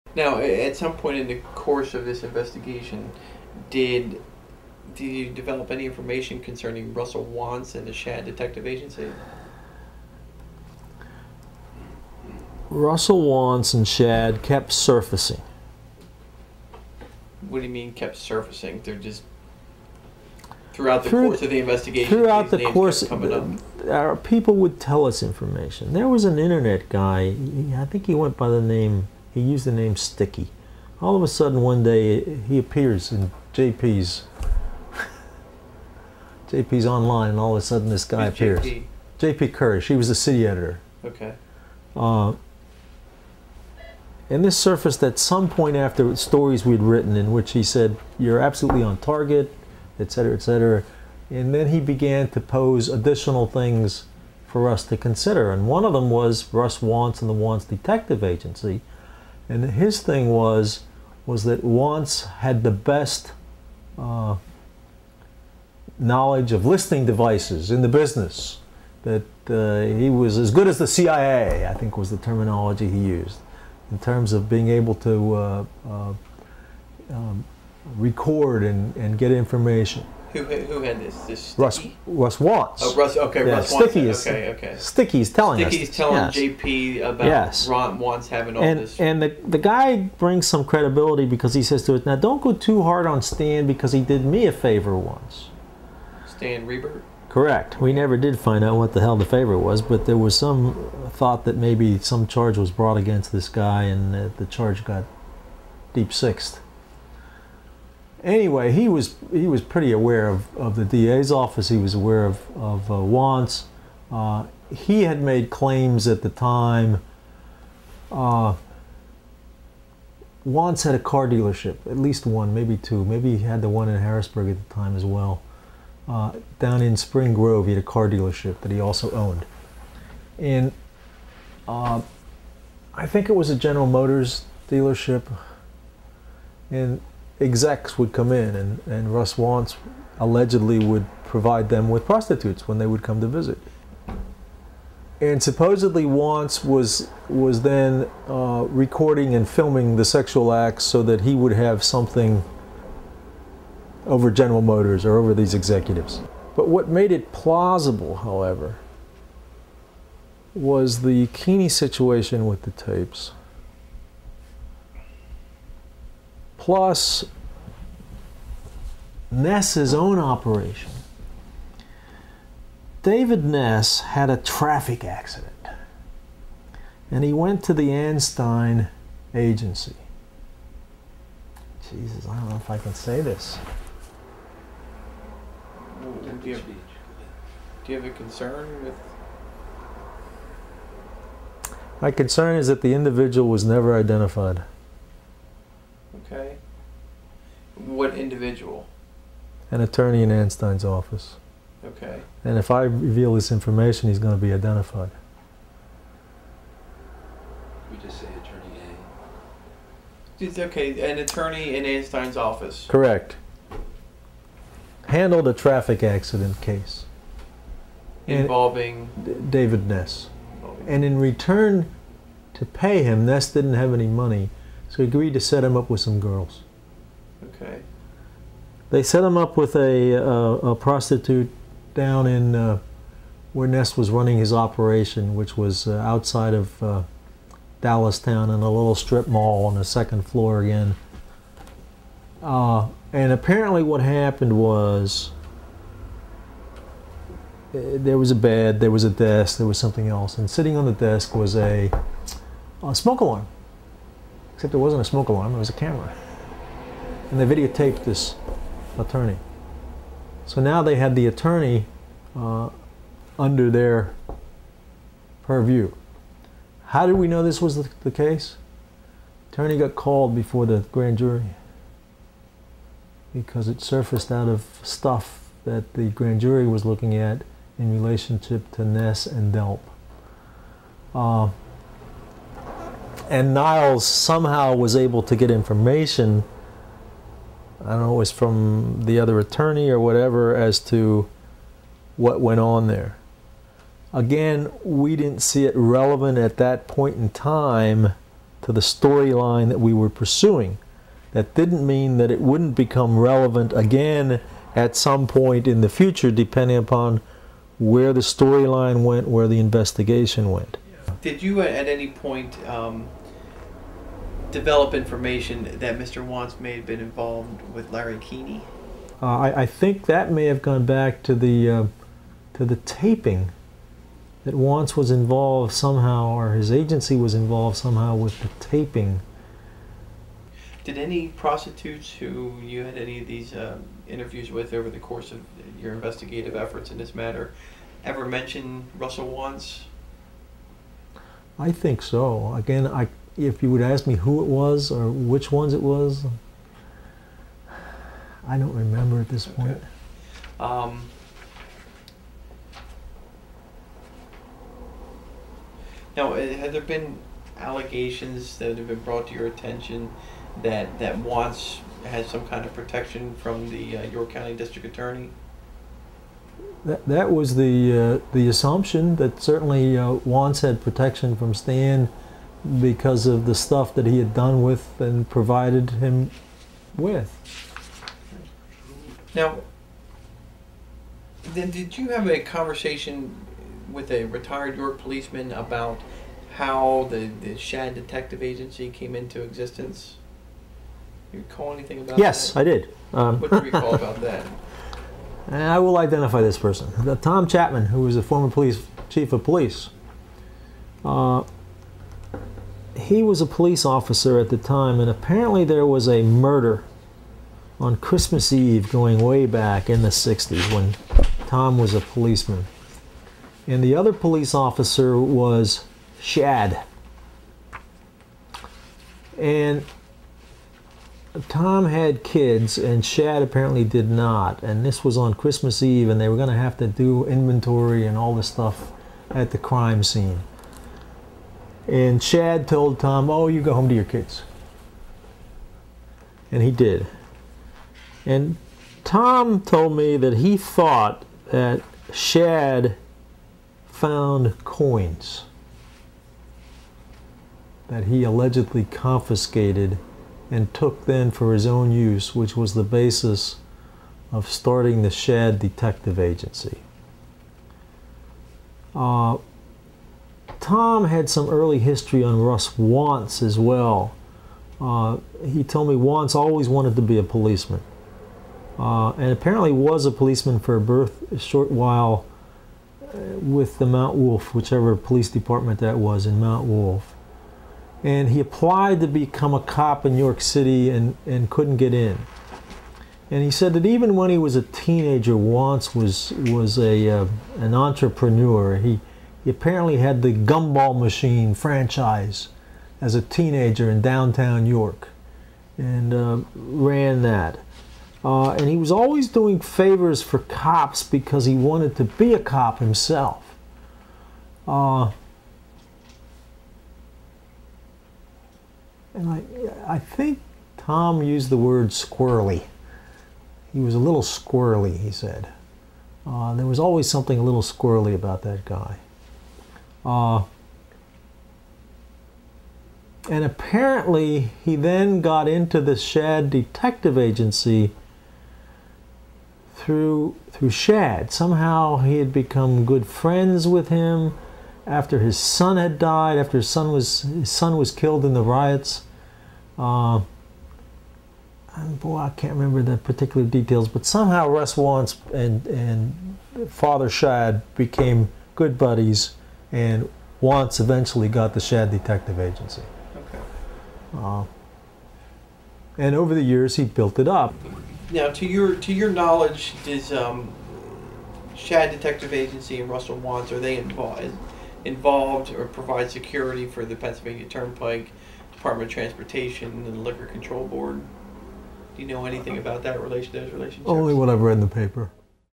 York County Pennsylvania courthouse sex ring Part 5
Part 5: "Russell Wantz and the Schaad Detective Agency." From an August 16, 2006 federal court video deposition involving the proposed banning of The Midnight Ride of Jonathan Luna, by William Keisling, and published by Yardbird Books. In this video, York, Pennsylvania city controller James Sneddon describes, under oath, a sex ring run from the York, PA county courthouse, and the alleged involvment of Russell Wantz, owner of the Schaad Detective Agency.